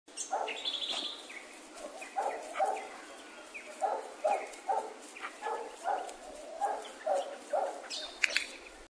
Kajsa stationary slalom
Kajsa lewg weaving -stationary

Slalom miedzy nogami -przewodnik stoi w miejscu